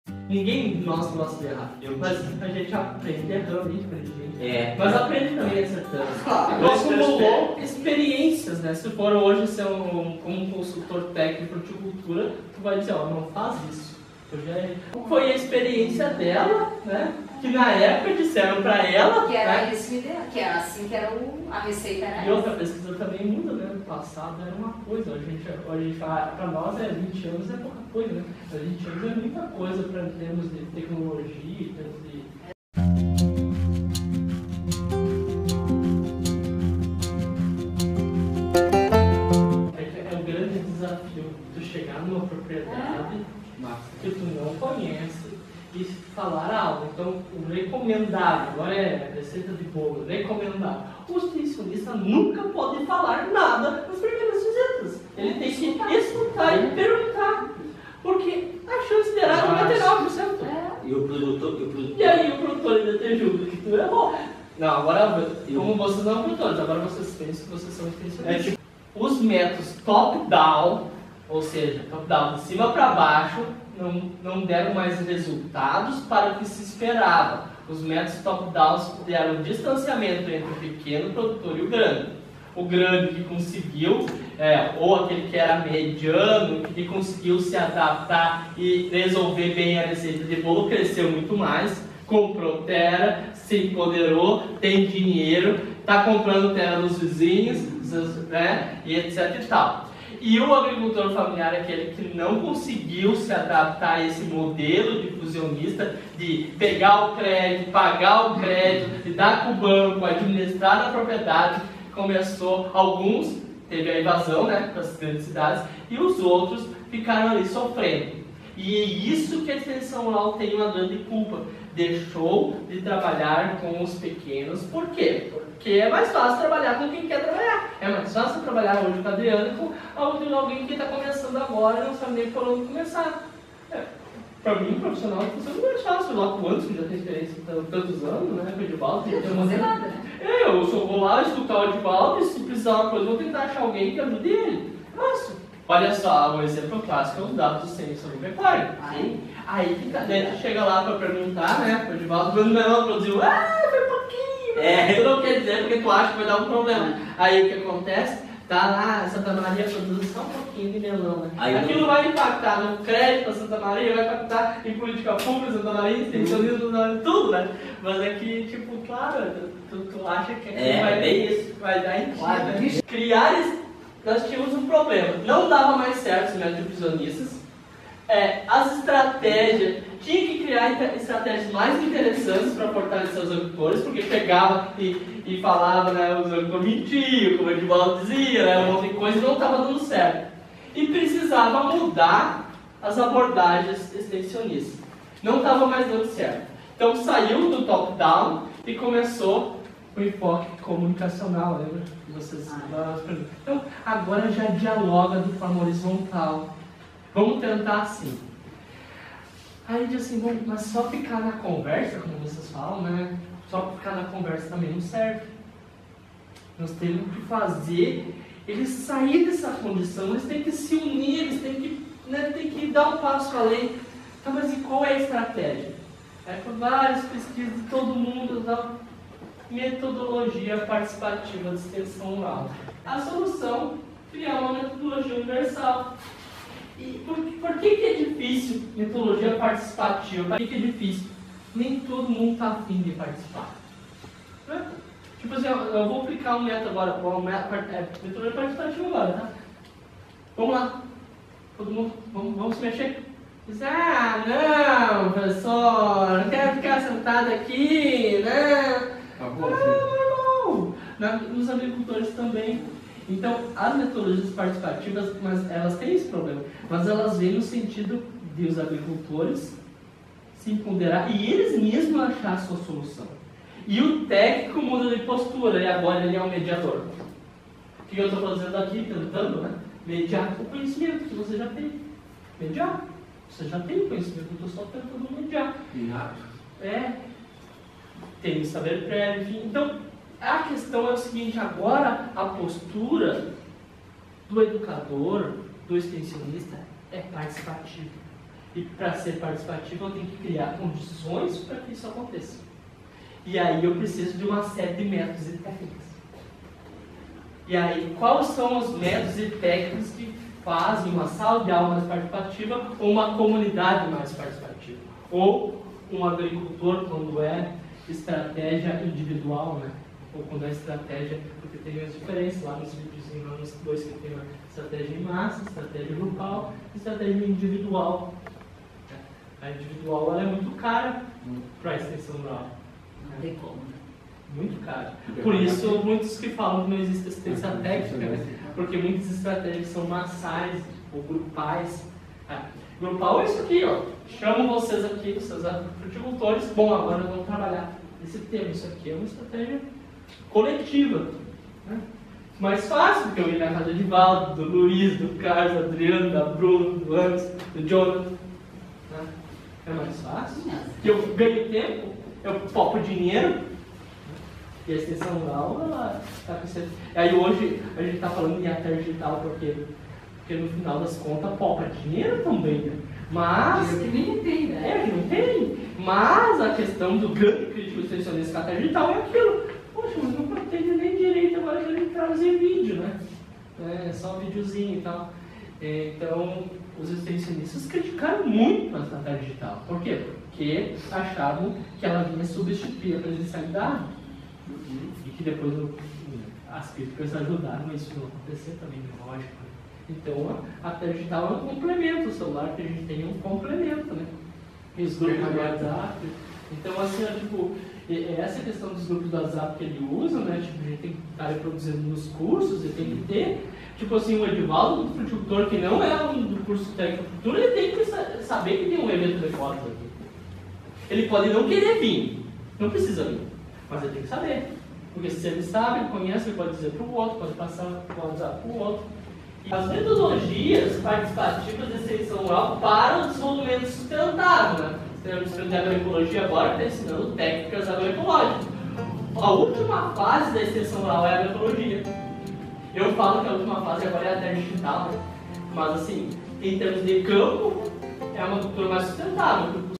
👉 Erros e acertos da Extensão Rural – o comportamento histórico do Extensionista Rural em jogo 💚
✒️Nesse vídeo refletimos, de forma rápida, com alguns exemplos práticos a evolução histórica das abordagens e dos enfoques da Extensão Rural. De uma abordagem difusionista para uma abordagem mais dialógica-comunicacional até chegar nas metodologias participativas de Extensão Rural.

Com erros, acertos e ajustes a Extensão Rural foi ganhando corpo e forma ao longo do tempo no Brasil. O vídeo apresenta algumas reflexões com os discentes do Curso Técnico em Fruticultura da UFSM. 🎓

Assista 😉😉

📚 Referência
RUAS, E. D. et al. Metodologia participativa de extensão rural para o desenvolvimento sustentável – MEXPAR. Belo Horizonte, Barbara Bela Editora Gráfica/Emater, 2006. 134 p.

🎓 Prof. Ezequiel Redin

🖋 Editoração: Equipe do Portal O Extensionista